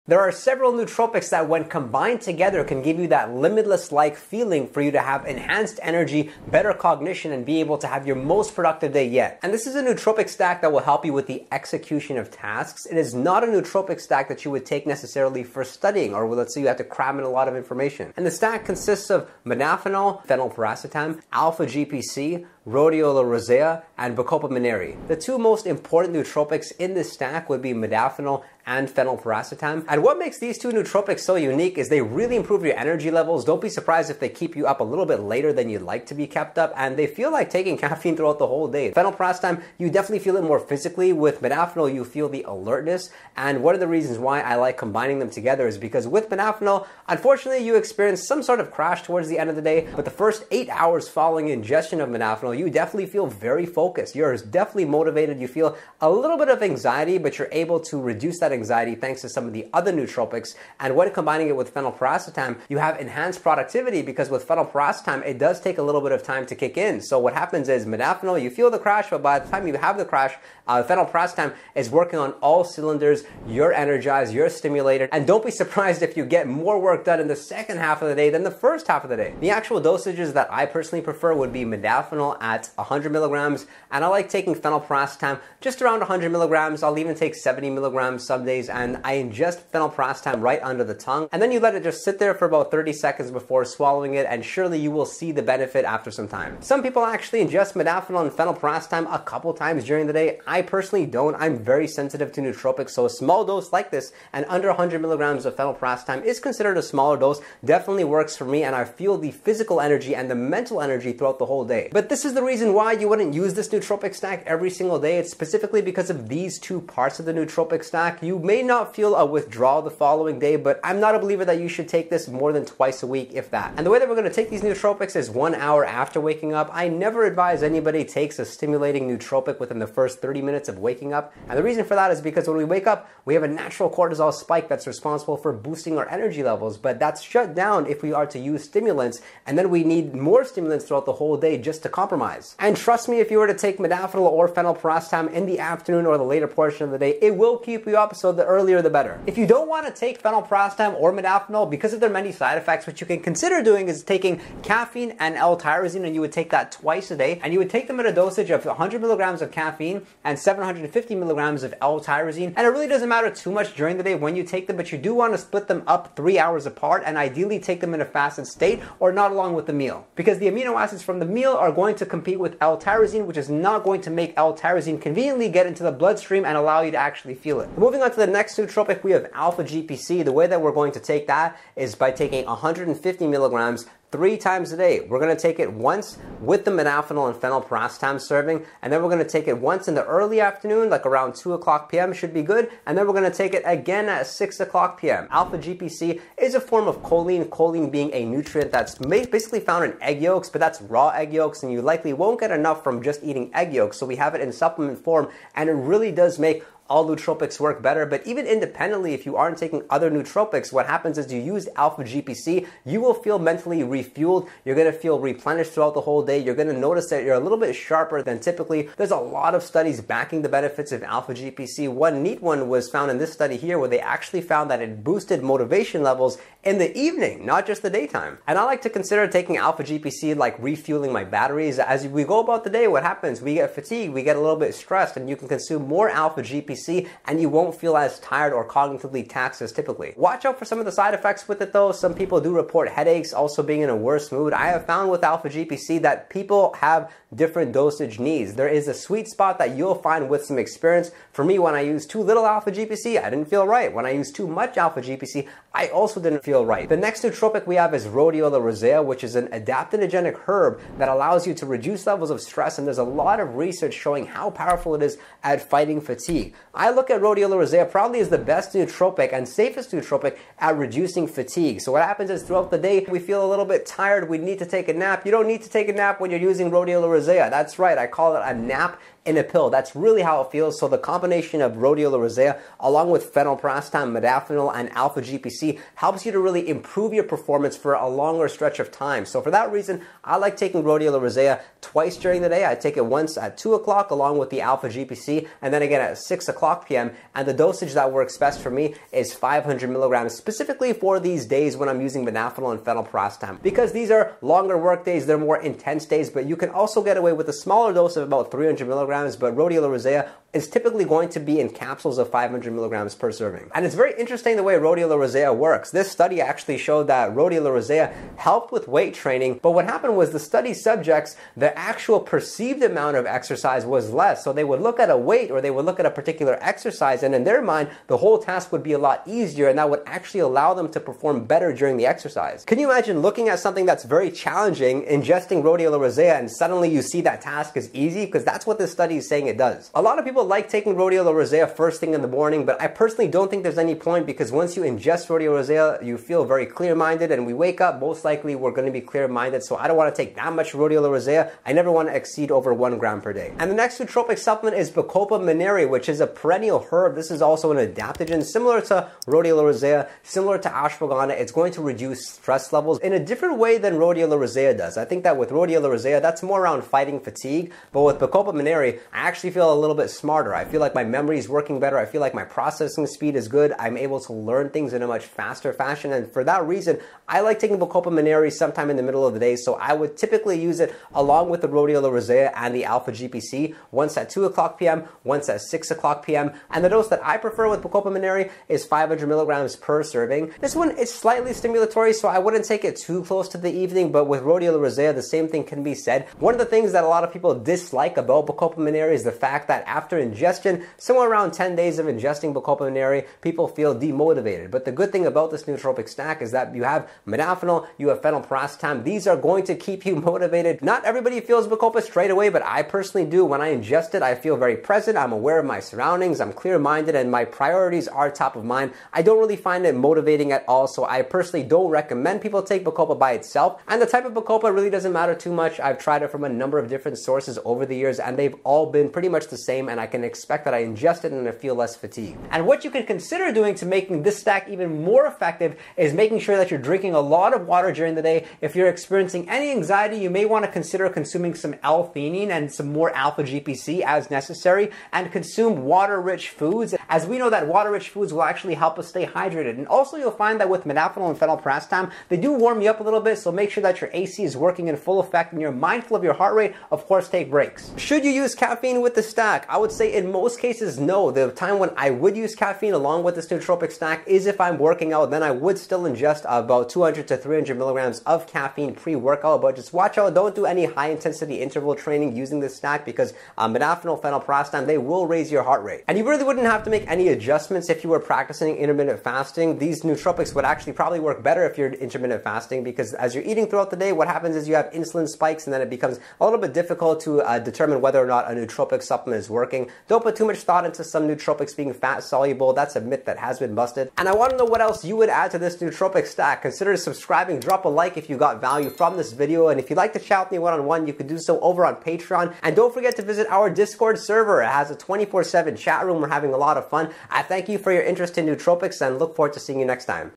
I Found The Real Life Limitless Nootropic Stack
Brace yourself as we unveil the ultimate real-life limitless nootropic stack, meticulously designed to unlock your mind's full potential. This stack promises to supercharge your energy, elevate your cognition, and skyrocket your productivity like never before.

⏰TIMESTAMPS⏰
00:00 Introduction
02:04 Can I Take Stimulants Right After Waking Up?
04:17 Adaptogen Herb For Anxiety
05:54 Adaptogen Herb For Memory
07:38 Should You Take Caffeine With This Stack?

Onnit
➡️ Try a FREE 15-day supply of Alpha BRAIN (VALID in USA only)